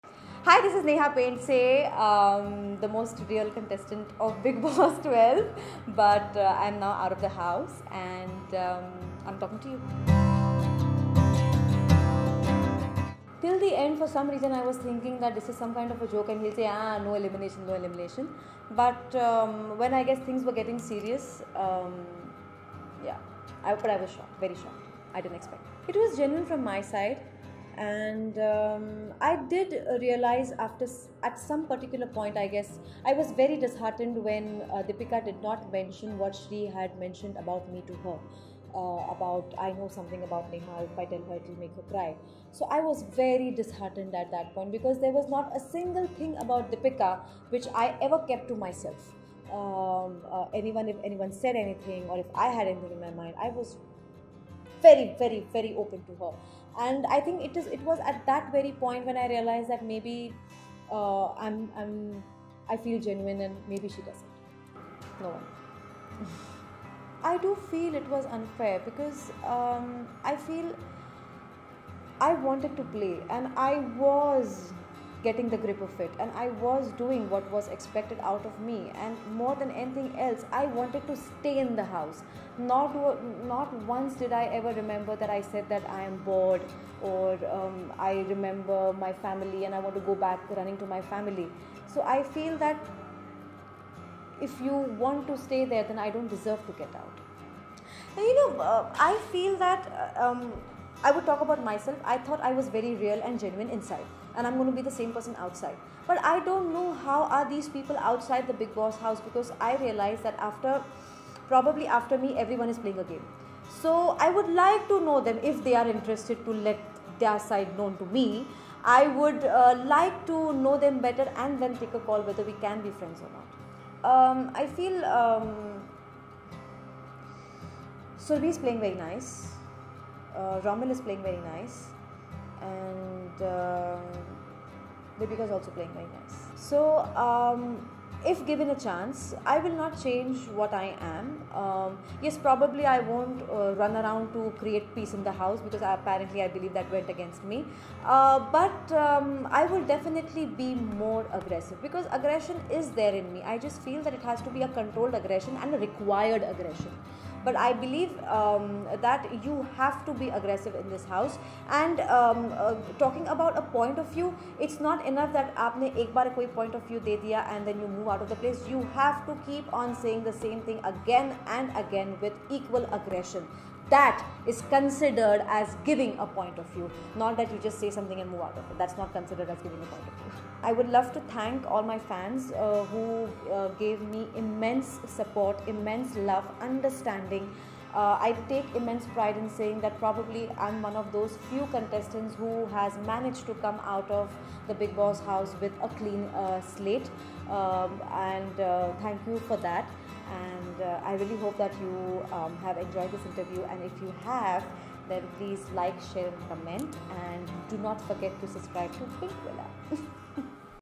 Bigg Boss 12 evicted contestant Nehha Pendse says, ‘I was heartbroken because of Dipika Kakar’
Bigg Boss 12 evicted contestant Nehha Pendse is as surprised as us with her eviction his week. In a candid conversation with Pinkvilla she told us if she repents the friendship with Dipika Kakar and if she would still remain friends with Sreesanth. She also revealed being heartbroken because of Dipika and if she would like to re enter the house.